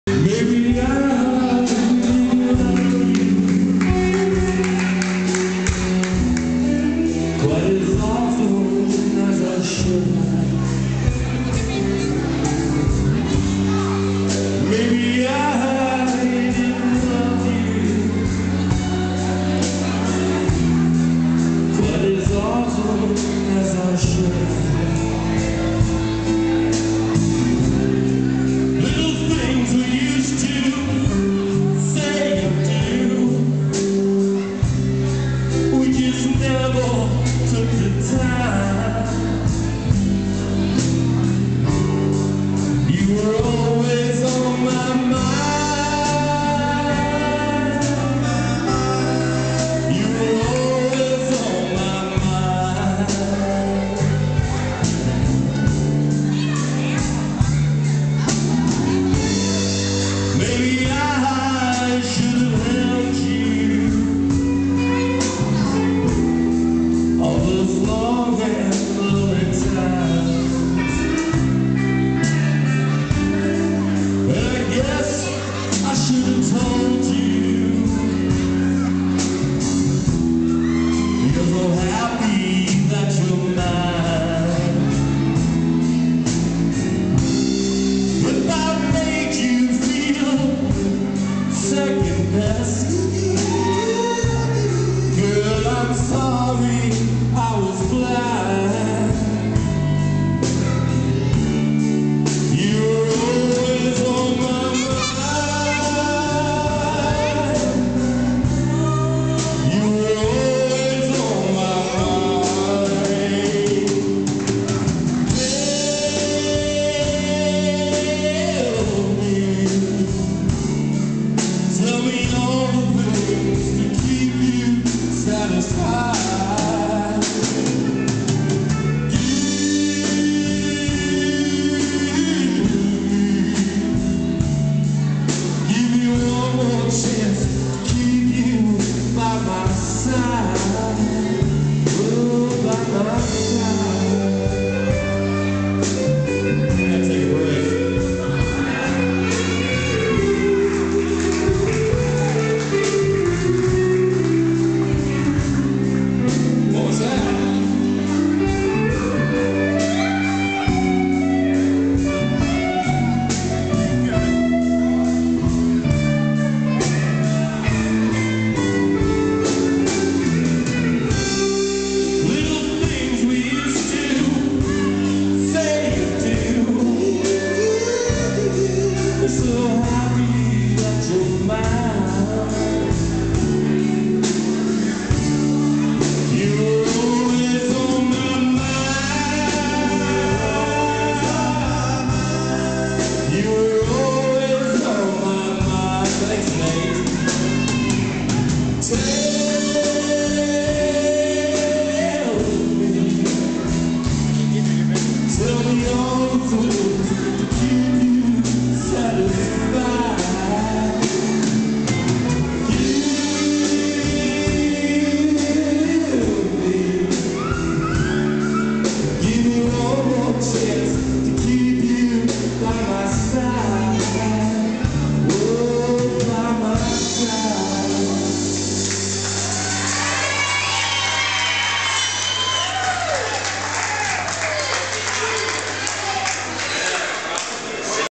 mika and james dance to Elvis @ wedding
Dancing to live Elvis at Wedding reception.